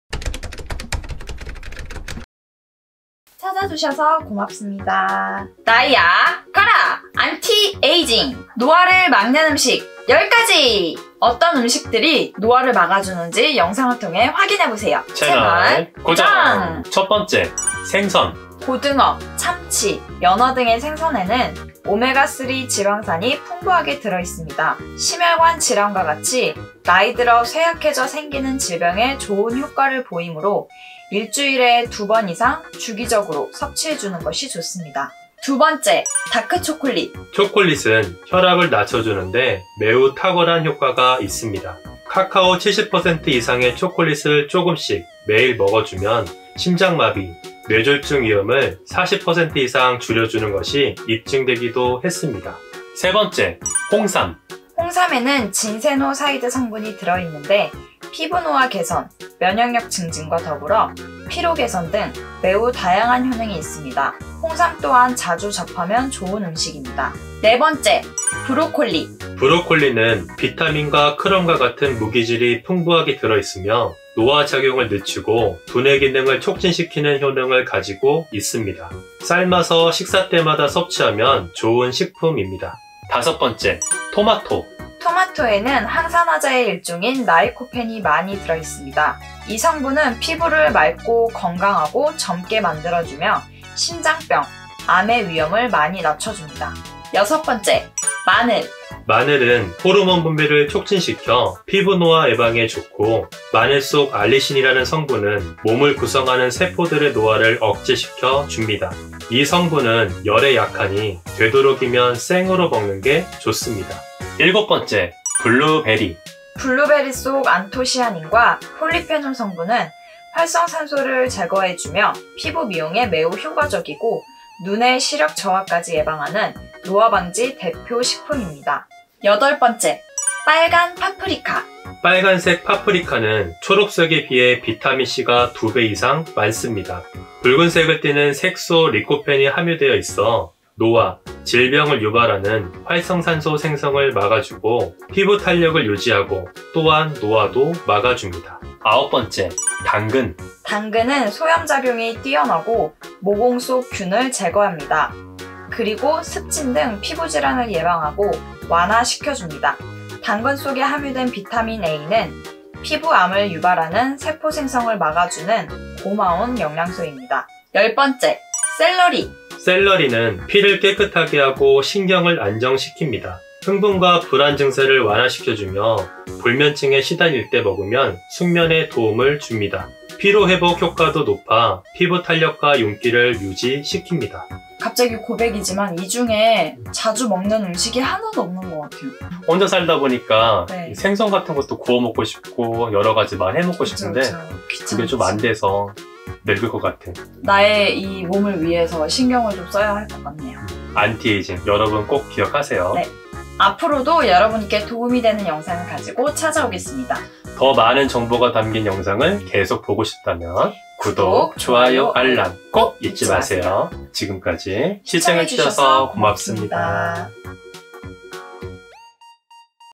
나이야 가라! 안티에이징! 노화를 막아주는 음식들 10가지
어떤 음식들이 노화를 막아줄까?